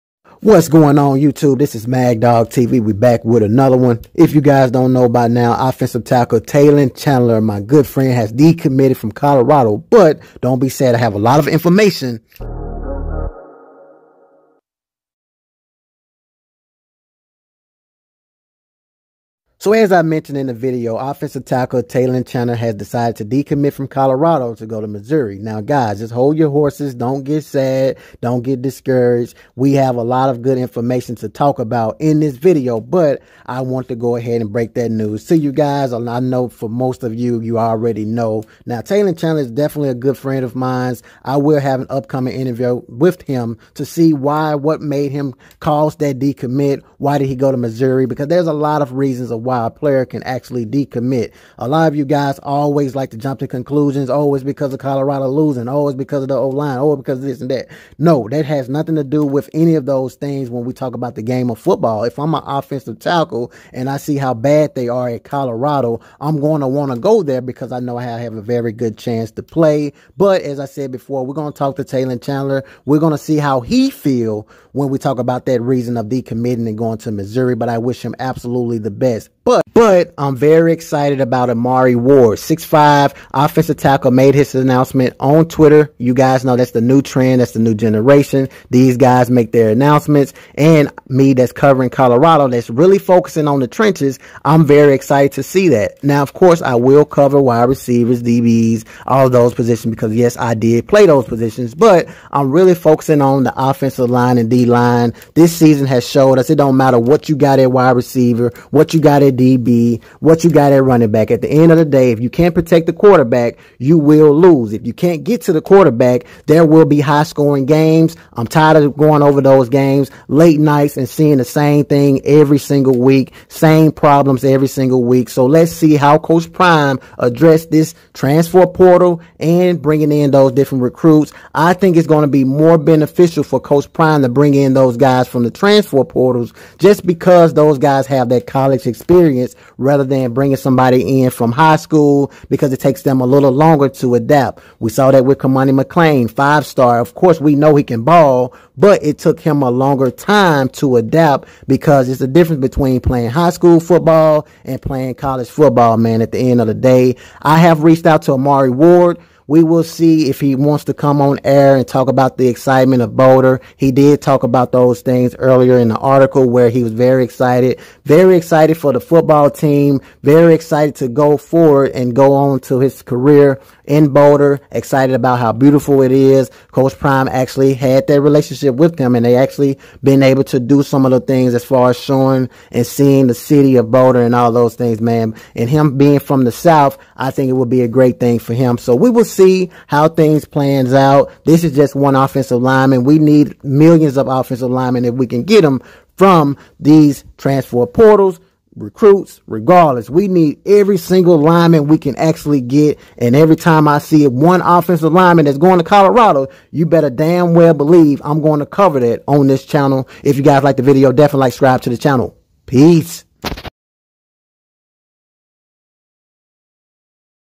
6'5 OT Amari Ward Is Going VIRAL For Making This Announcement : "I'm Ready, Coach "
Colorado Buffs football team will prepare for its final game of the Season versus Utah.  There's a good chance Buffs will lose due to the OLine & DLine.

   Coach Prime must address the trenches in order to sustain another let down in the Big 12 next year. in this video,  I cover 6'5 OT Amari Ward!